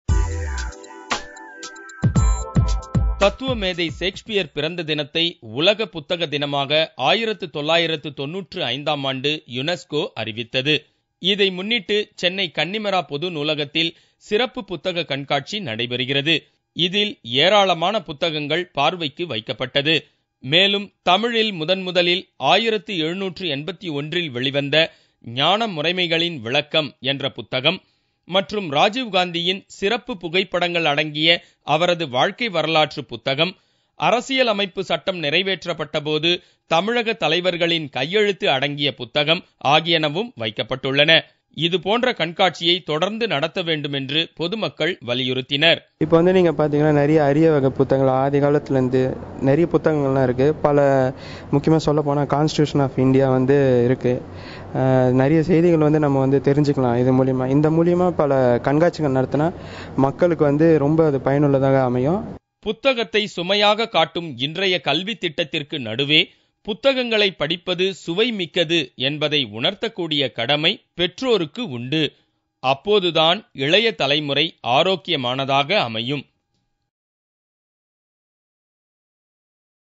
23 April-World book day
To celebrate World book day, Connemara library has organised a book exhibition. Many books are exhibited here. It is always true that books are best soul mate.